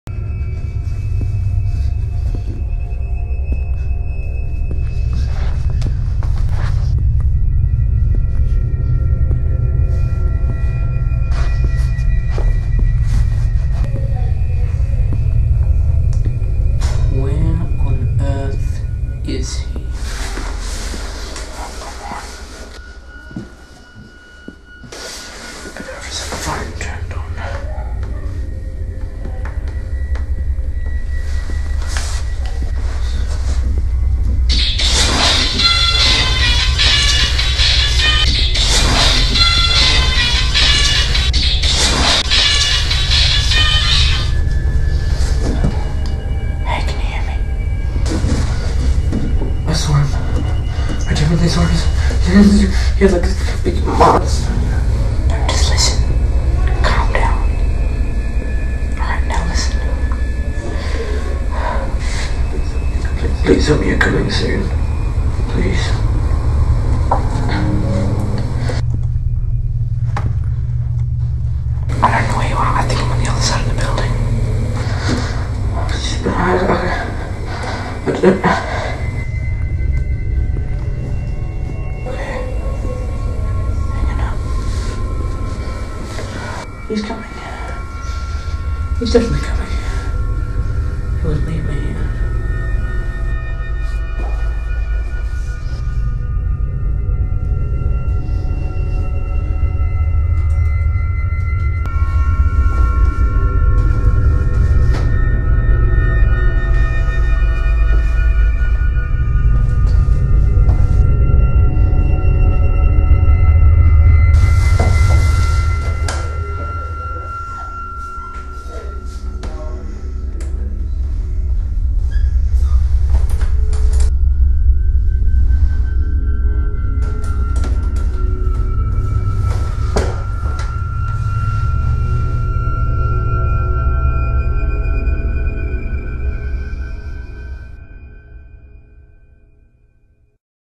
Short Film Test (Horror?)
Just a short test.
it's not meant to look 100% Proffesional
I was just testing out a few things.
will be working on a more proffesional film as soon as an idea pops into my head XD